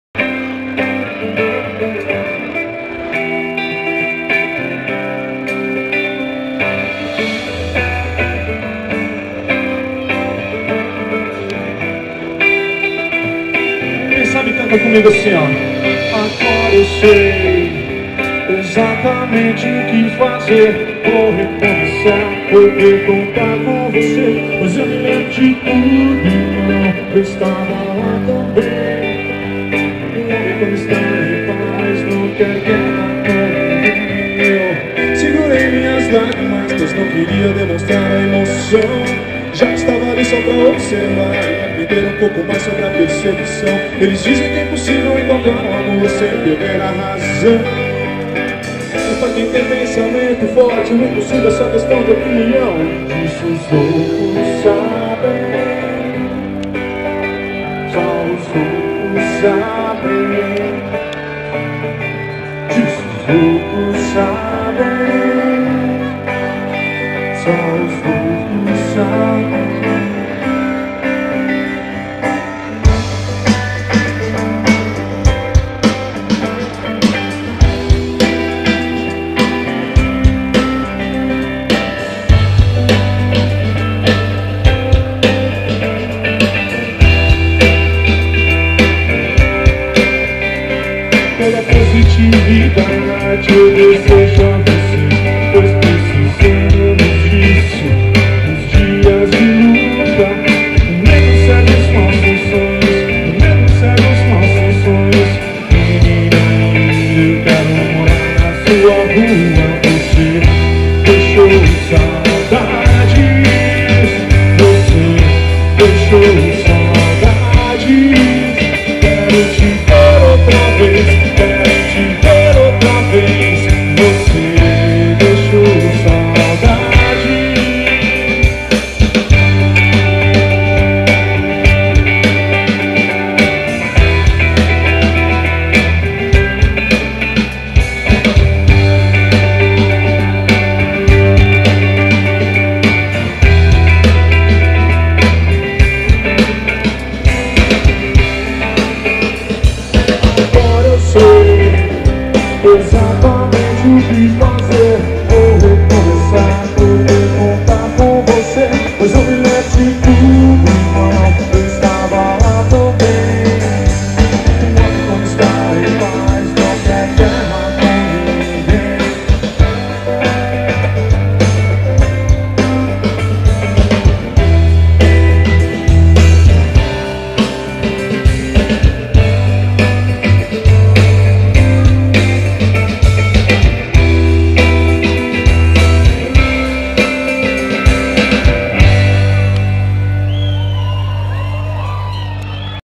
BANDA ROKELLME (18º ENCONTRO DE MOTOCICLISTAS DE ARTUR NOGUEIRA P7
DOMINGO 27/04/14, SHOW COM A BANDA ROKELLME ,NO ENCONTRO DE MOTOS DE ARTUR NOGUEIRA 2014
ARTUR NOGUEIRA CIDADE MAIS BONITA DO MUNDO